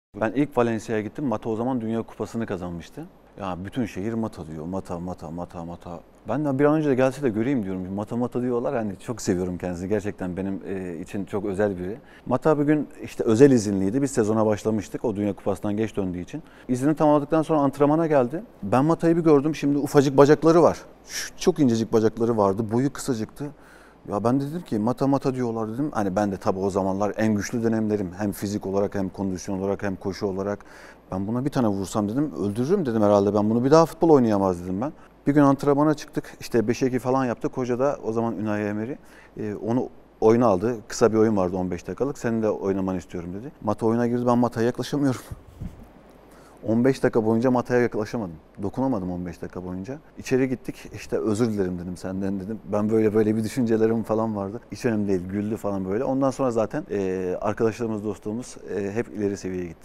Mehmet Topal'ın Mata Anısı "Ben buna bir tane vursam öldürürüm..." 😂
💬 Mehmet Topal'ın Juan Mata anısı: 'Ben buna bir tane vursam öldürürüm bir daha futbol oynayamaz' dedim.

Programın tamamını kanalımızdan izleyebilirsiniz.